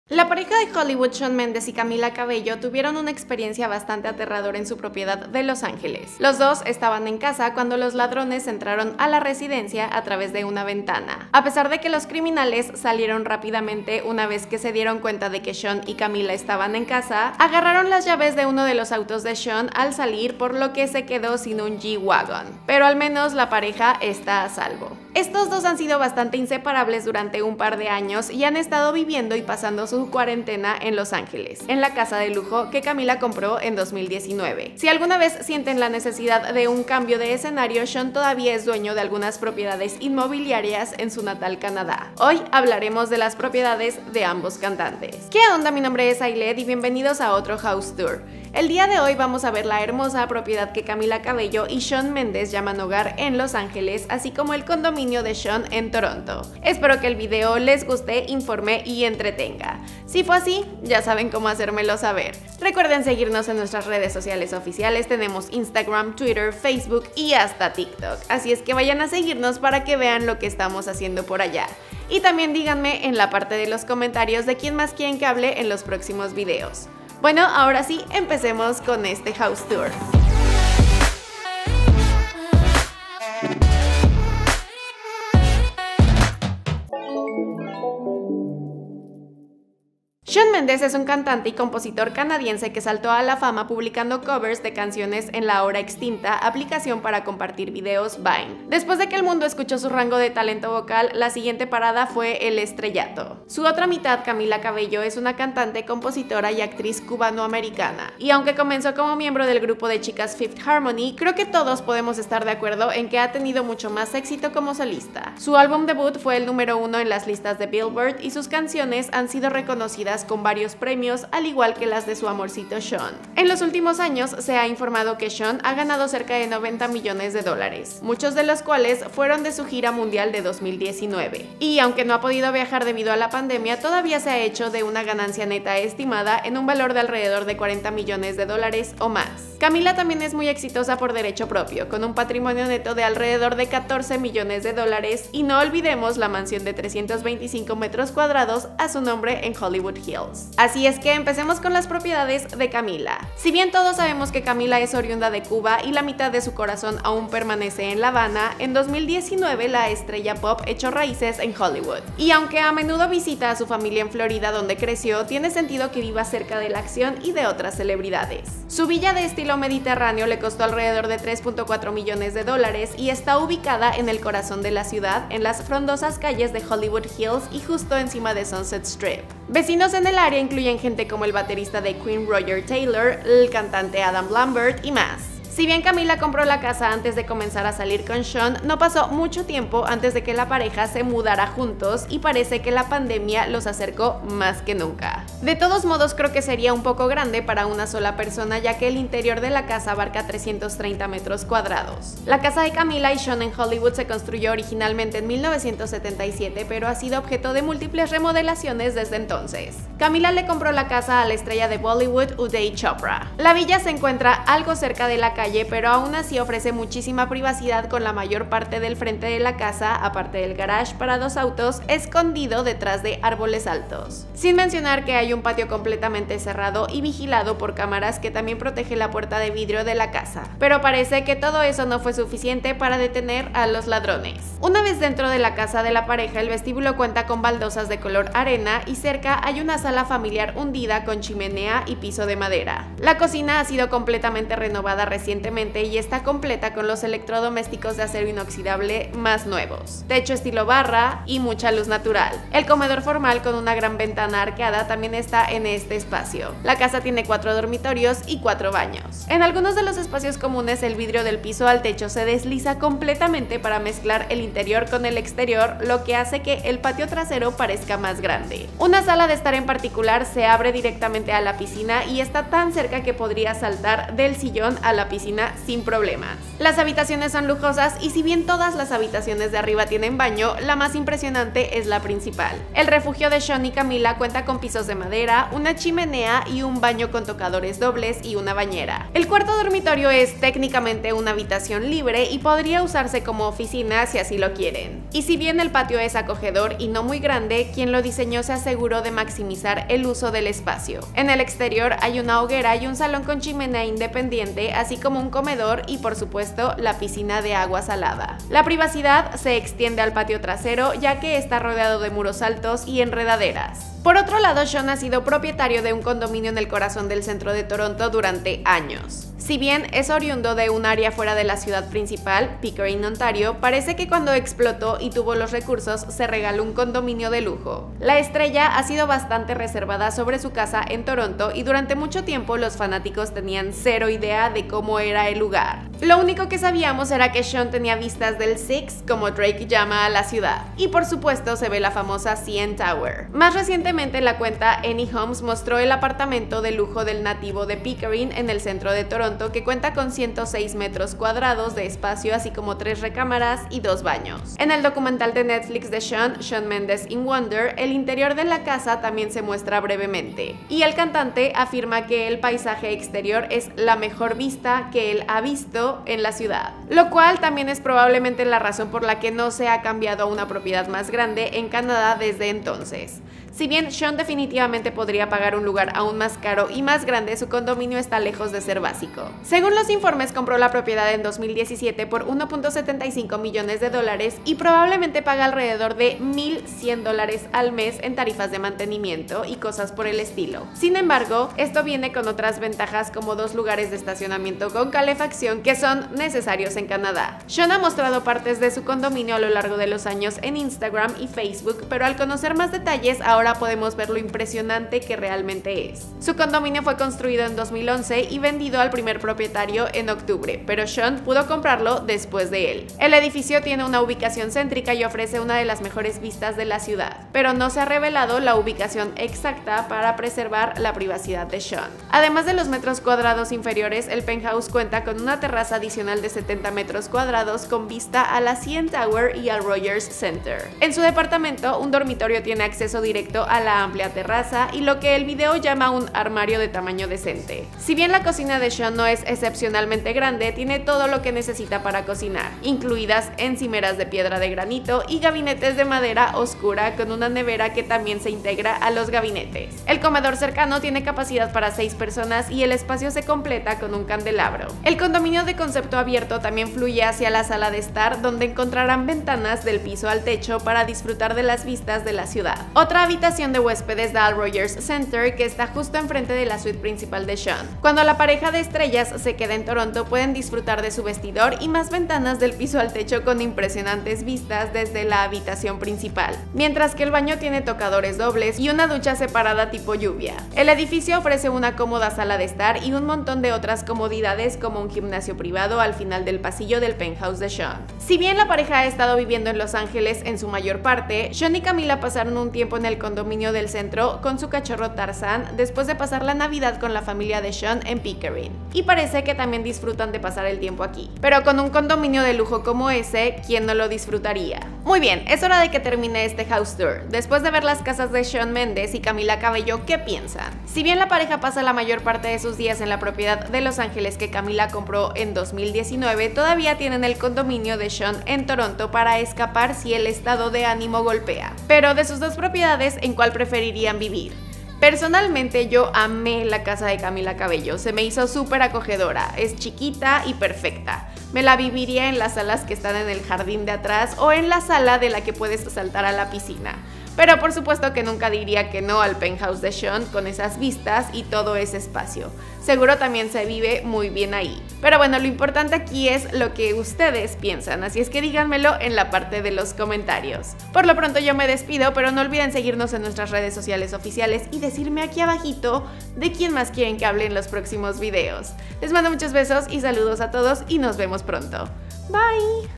Shawn Mendes Y Camila Cabello | House Tour | Lujosa mansión en Los Ángeles & Toronto 🏡
Shawn Mendes es un cantante Y compositor canadiense que saltó a la fama publicando COVERS de canciones en la ahora extinta aplicación para compartir videos, Vine. Después de que el mundo escuchó su rango de talento vocal, la siguiente parada fue el estrellato.Su otra mitad, Camila Cabello es una cantante, compositora y actriz cubano americana. Y aunque comenzó como miembro del grupo de chicas Fifth Harmony, creo que todos podemos estar de acuerdo en que ha tenido mucho más éxito como solista.